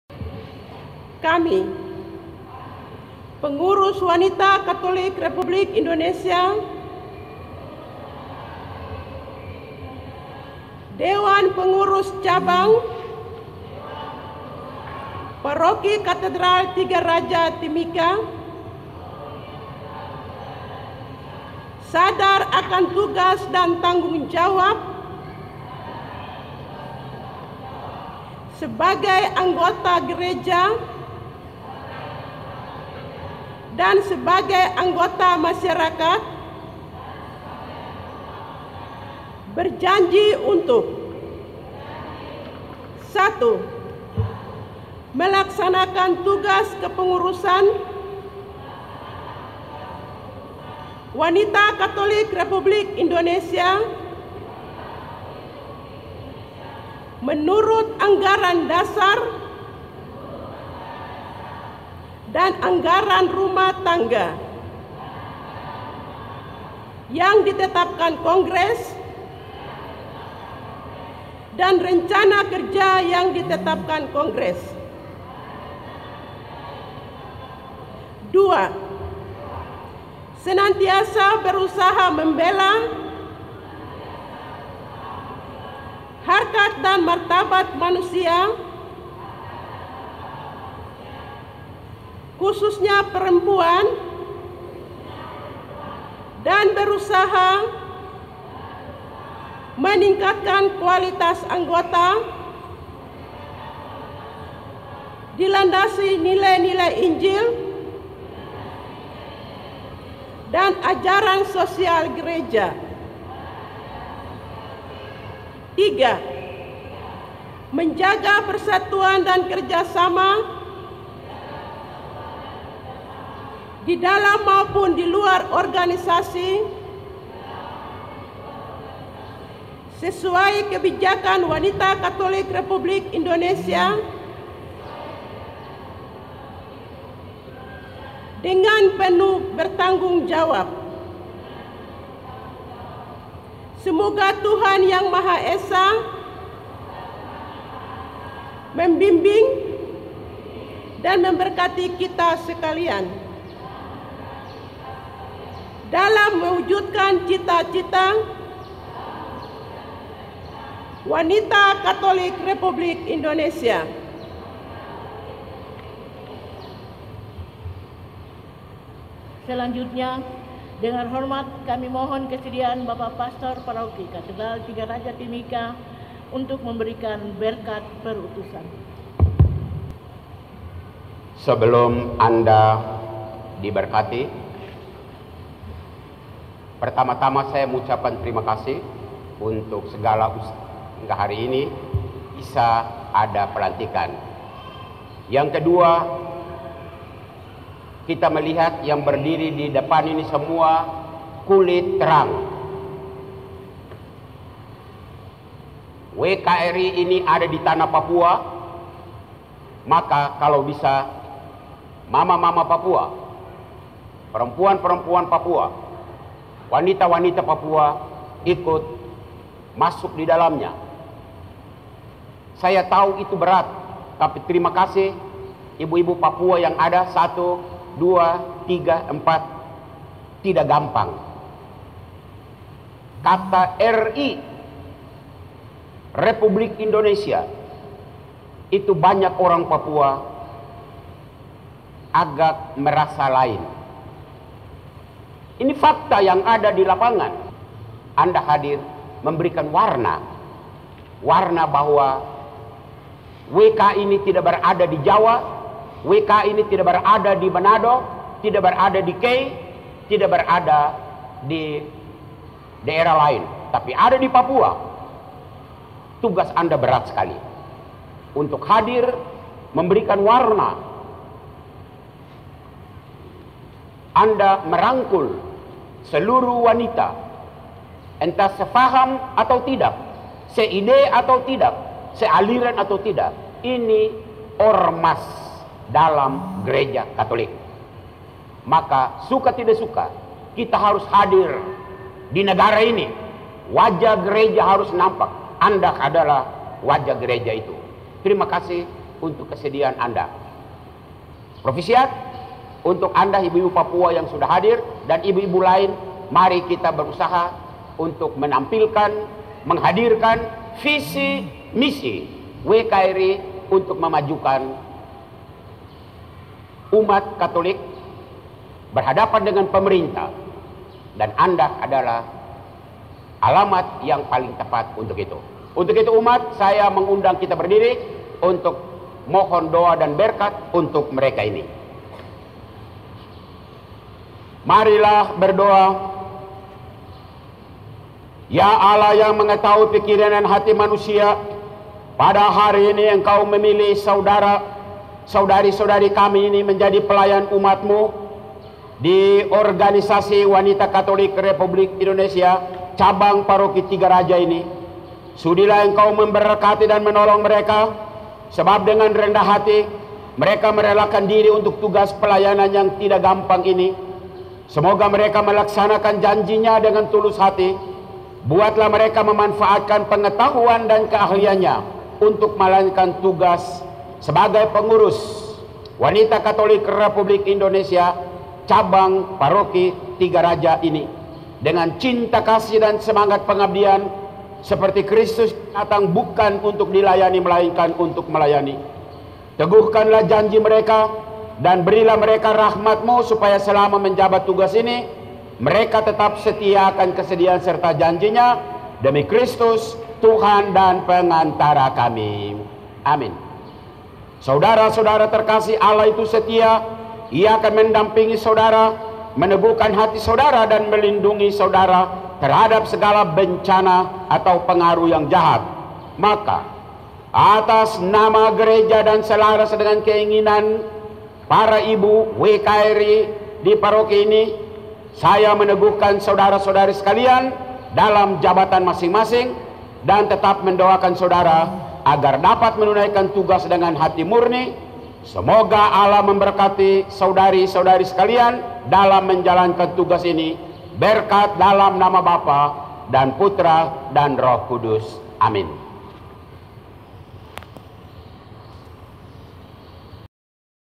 Janji WKRI & Berkat Perutusan
Dalam acara ini kaka Fransina Belau turut dilantik sebaga wakil ketua.